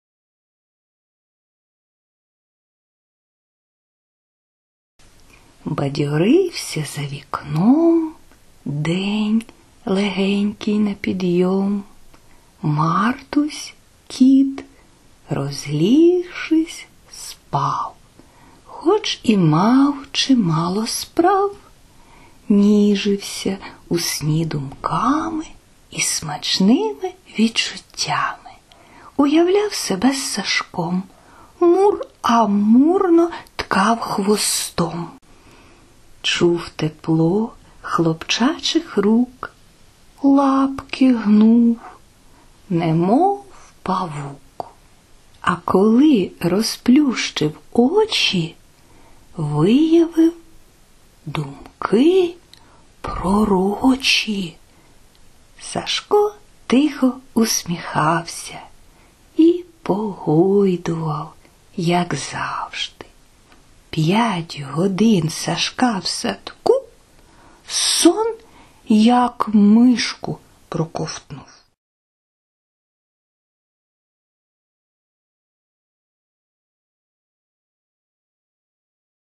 'The Dream For Happiness'(in Ukrainian)/ 'Сон у лапку'
Videopoem for children 'The Dream For Happiness' / Відеовірш для дітей 'Cон у лапку'
Painter - Olesia Dzhurayeva / Художник - Олеся Джураєва,
Text - Halka Myr / Текст - Галка Мир